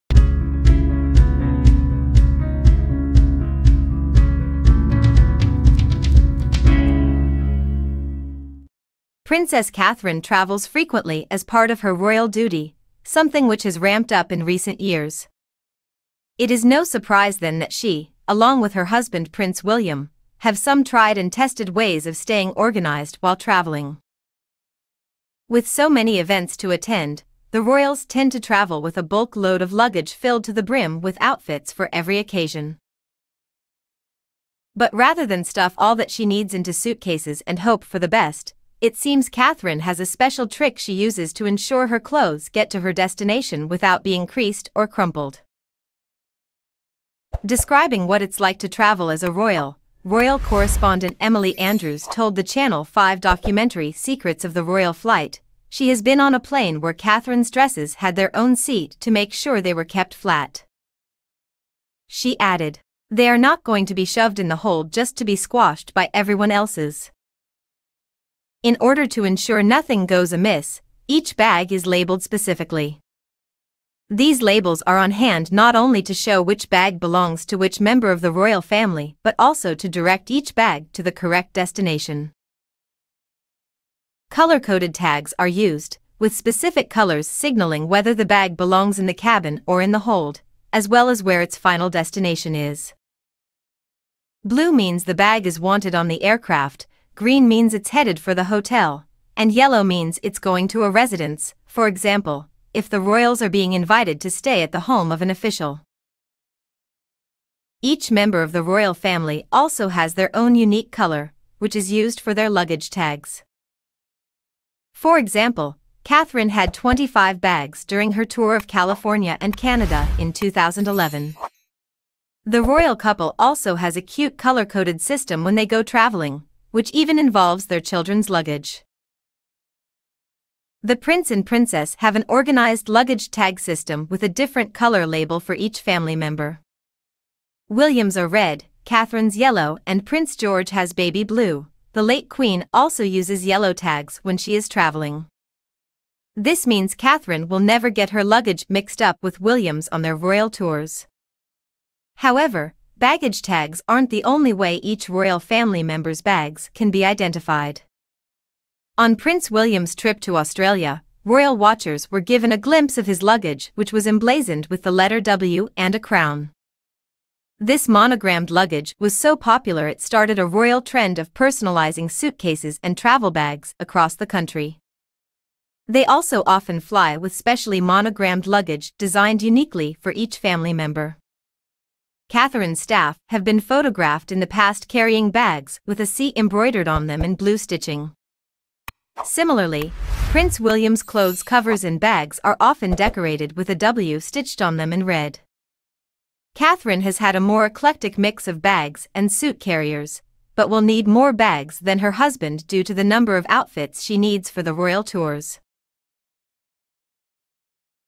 Secrets William And Catherine's Clever Luggage Packing Trick During The Royal Tour Revealed
Secrets William And Catherine's Clever Luggage Packing Trick During The Royal Tour Revealed @InsideRoyalLife

Check out this not-to-be-missed video: William And Catherine Surprises Fellow Passengers On Commercial British Airways Flight